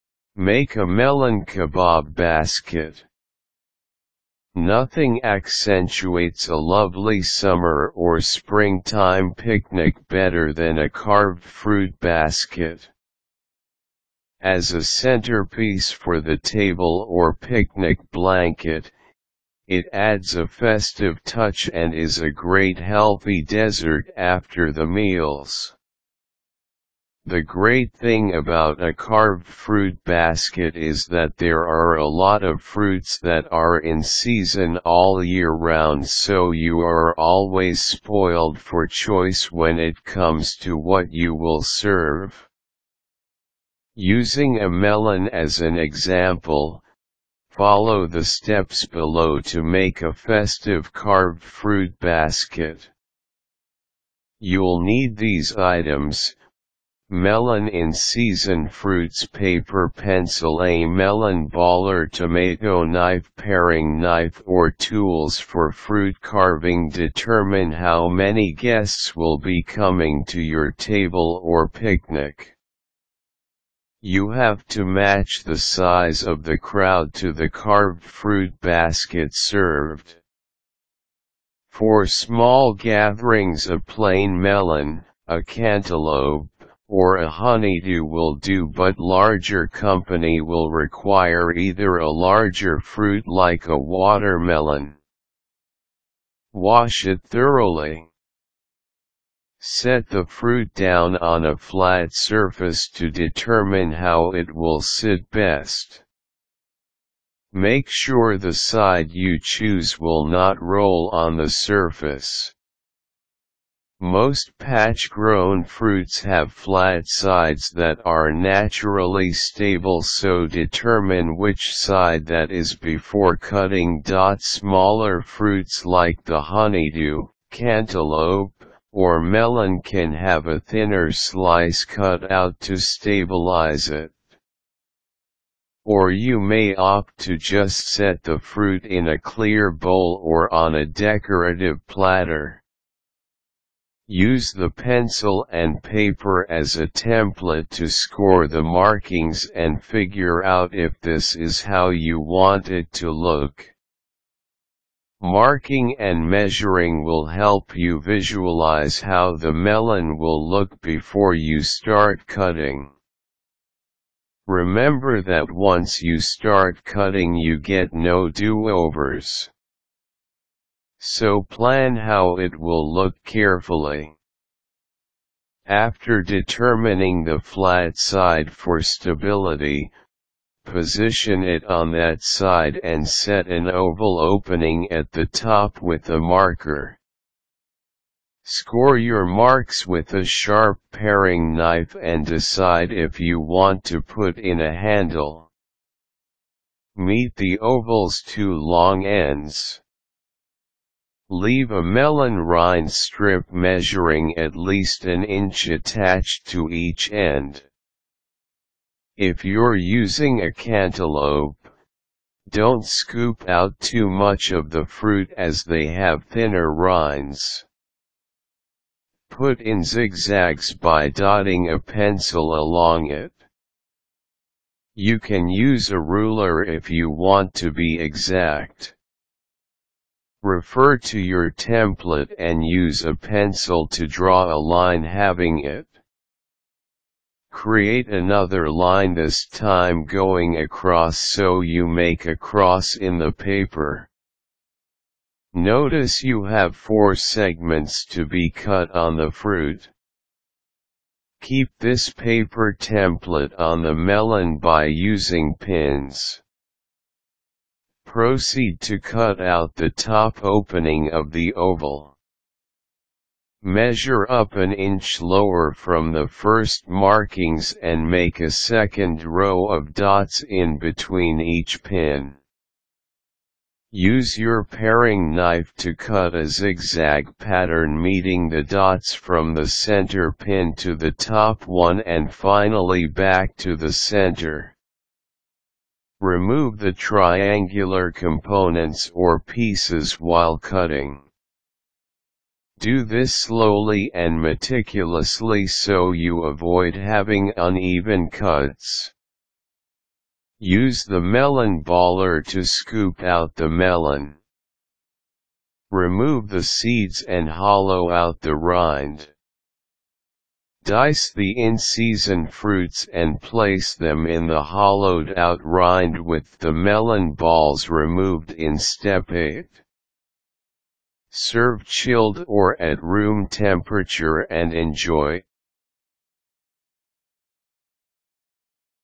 How to : Make a melon kebob basket
Make a melon kebob basket

 Nothing accentuates a lovely summer or springtime picnic better than a carved fruit basket. As a centerpiece for the table or picnic blanket, it adds a festive touch and is a great healthy desert after the meals. The great thing about a carved fruit basket is that there are a lot of fruits that are in season all year round so you are always spoiled for choice when it comes to what you will serve.

Using a melon as an example, follow the steps below to make a festive carved fruit basket.

 You’ll need these items:

 Melon

In season fruits

Paper

Pencil

A melon baller

Tomato knife

Paring knife or tools for fruit carving Determine how many guests will be coming to your table or picnic. You have to match the size of the crowd to the carved fruit basket served. For small gatherings a plain melon, a cantaloupe or a honeydew will do but larger company will require either a larger fruit like a watermelon. Wash it thoroughly. Set the fruit down on a flat surface to determine how it will sit best. Make sure the side you choose will not roll on the surface. Most patch-grown fruits have flat sides that are naturally stable so determine which side that is before cutting.Smaller fruits like the honeydew, cantaloupe or melon can have a thinner slice cut out to stabilize it. Or you may opt to just set the fruit in a clear bowl or on a decorative platter. Use the pencil and paper as a template to score the markings and figure out if this is how you want it to look. Marking and measuring will help you visualize how the melon will look before you start cutting. Remember that once you start cutting you get no do-overs. So plan how it will look carefully. After determining the flat side for stability, position it on that side and set an oval opening at the top with a marker. Score your marks with a sharp paring knife and decide if you want to put in a handle. Meet the oval’s 2 long ends. Leave a melon rind strip measuring at least an inch attached to each end. If you’re using a cantaloupe, don’t scoop out too much of the fruit as they have thinner rinds. Put in zigzags by dotting a pencil along it. You can use a ruler if you want to be exact. Refer to your template and use a pencil to draw a line halving it. Create another line this time going across so you make a cross in the paper. Notice you have four segments to be cut on the fruit. Keep this paper template on the melon by using pins. Proceed to cut out the top opening of the oval. Measure up an inch lower from the first markings and make a 2nd row of dots in between each pin. Use your paring knife to cut a zigzag pattern meeting the dots from the center pin to the top one and finally back to the center. Remove the triangular components or pieces while cutting. Do this slowly and meticulously so you avoid having uneven cuts. Use the melon baller to scoop out the melon. Remove the seeds and hollow out the rind.

 Dice the in-season fruits and place them in the hollowed out rind with the melon balls removed in step 8. Serve chilled or at room temperature and enjoy!